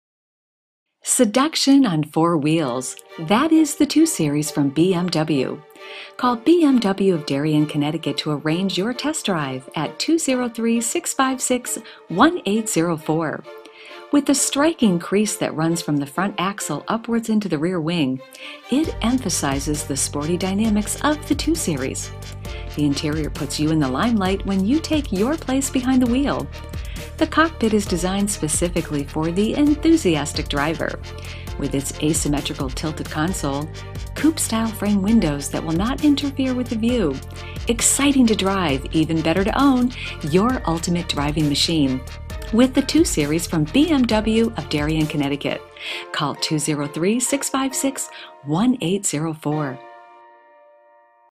BMW 2 Series Darien Connecticut
Seduction on four wheels, that is the 2 Series from BMW.

With the striking crease that runs from the front axle upwards in to  the rear wing, it emphasizes the sporty dynamics of the 2 Series.  The interior puts you in the limelight when you take your place behind the wheel.  The cockpit is designed specifically for the enthusiastic driver, with its  asymmetrical tilted console, coupe style framed windows that will not interfere with the view, exciting to drive and even better to own your Ultimate Driving Machine with the 2 Series from BMW of Darien Connecticut.

The BMW 2 Series in Darien Connecticut is leading the pack in turbo engines and power in the coupe class. The Audi and others just cannot compete.  The 2-series comes in at number one, for affordability and variety of style. For people from all walks of life, whether it’s the mountain climber looking for a quick escape, or the urban commuter, each has much to gain from BMW’s enduring quality.

Much of what you see from the competition is just more trunk space. But you won’t be driving from the back, least of all the trunk. The only place you should ever be in your car, when its moving at least, is in the driver’s seat.

The engine options do change, as there are two automatic options and one manual. In addition to that a different engine is offered with the coupe. But even the most basic engine will purr. It’s a one for all and all for one design. And underneath every model of this series is the signature sound a high quality German engine.

The upgrades make this series number one. The Real-Time Traffic updates keep you informed. And the online capabilities will keep you connected to all your Facebook and Twitter. Or turn your smartphone into a hub for Heads Up Display. The headlights corners and pivots with the car is an added safety feature, and definitely one that will stick around.

Keyless entry and a universal remote to open your garage door for you are two favorites among security conscious.

And just like muscle memory, the innovators at BMW came up with a way to adjust the suspension of your car according to what road conditions you most often encounter so as you drive, the smoothest and most enjoyable experience possible awaits. BMW 2 Series in Darien Connecticut, just miles from the Big Apple is a one stop shop for the local community.